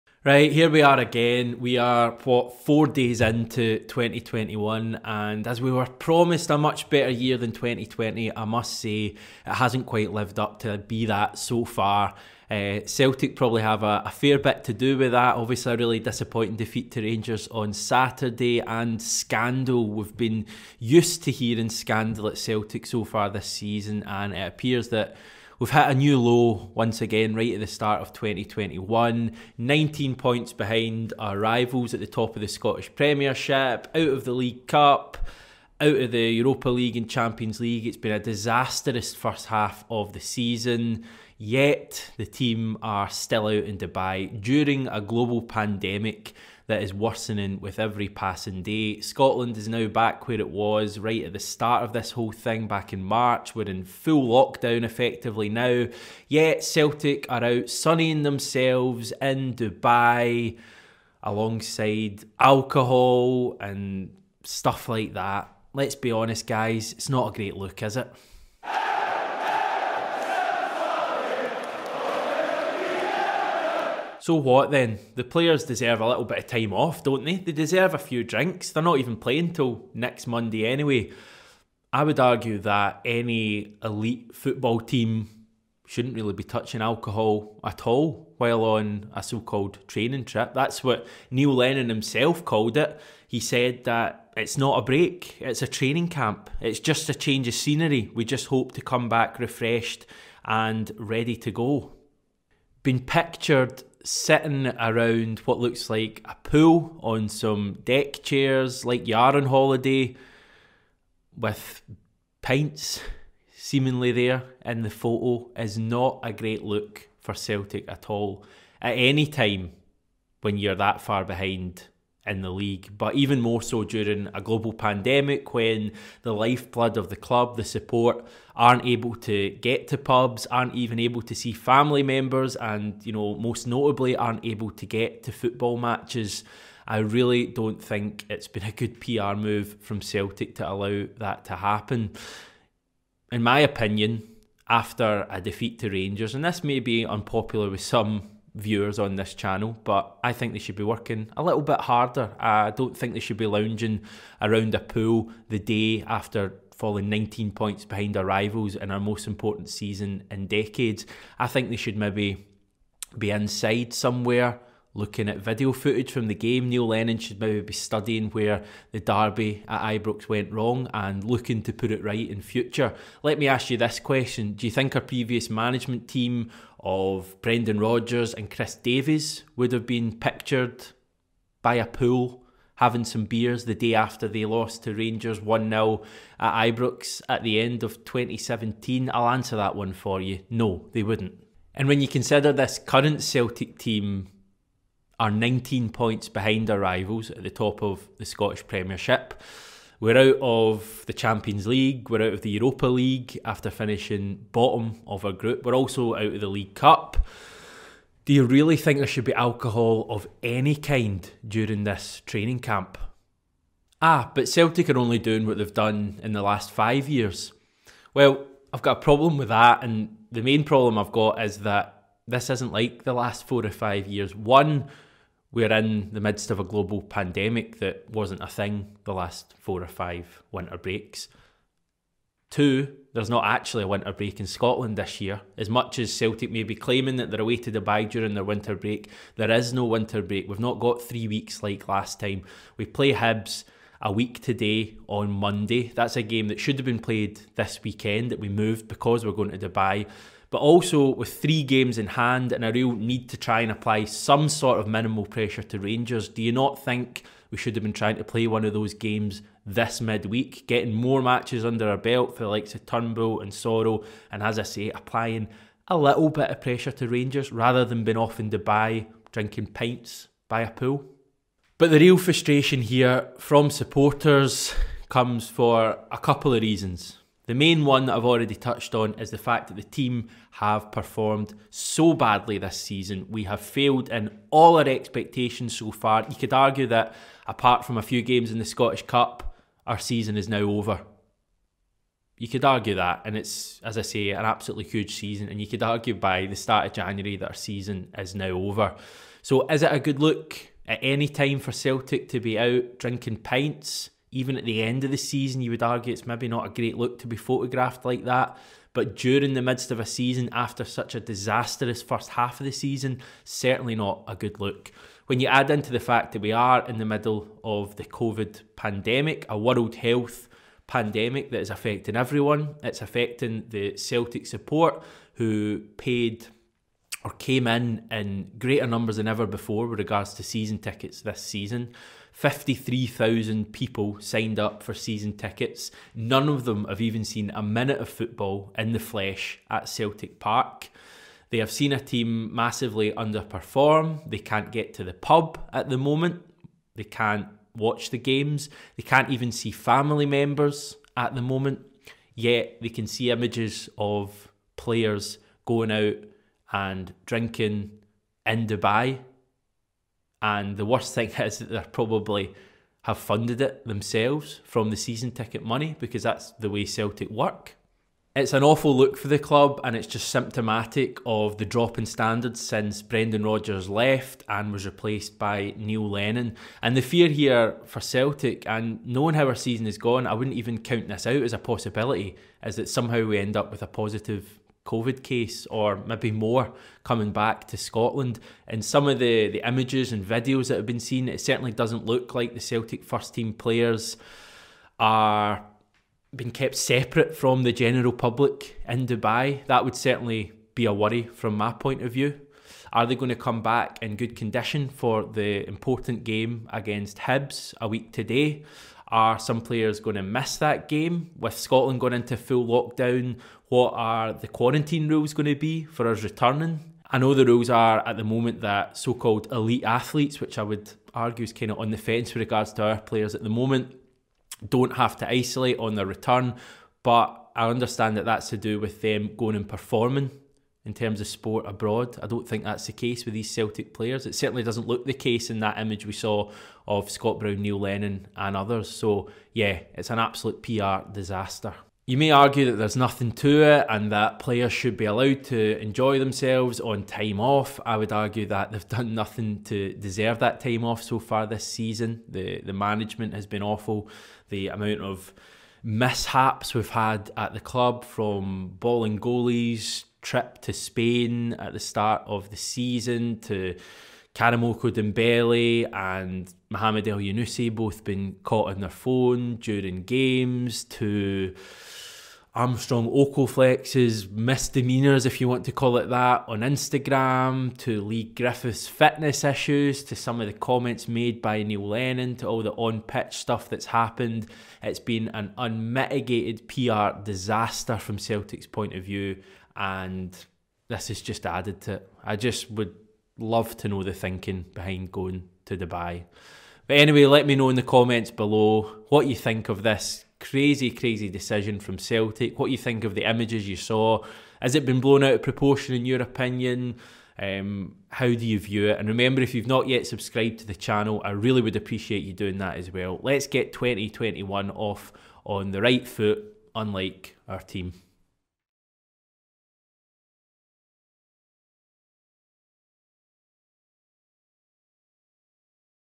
Celtic in Dubai: A major own goal
Hamish takes a look at Celtic's well documented trip to Dubai and the recent image that emerged of Neil Lennon and Scott Brown enjoying a drink by a pool.

Is this a good look for a team currently 19 points behind at the top of the Premiership and out of every other cup they've entered?